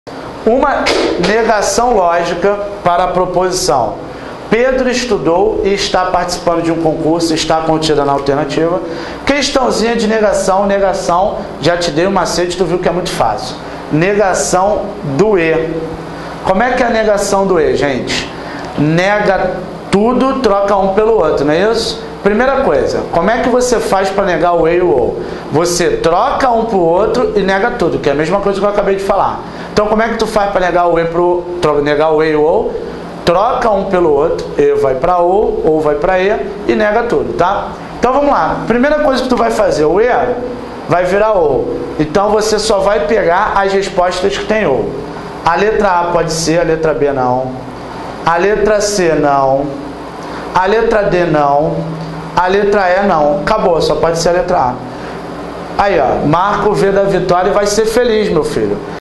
Raciocínio Lógico para INSS |Dica 12| Negação lógica ficou fácil
Você não sabe Raciocínio Lógico para o INSS ?

Então você precisa aprender com esse vídeo onde nosso professor, Renato, mostra Raciocínio Lógico para o INSS.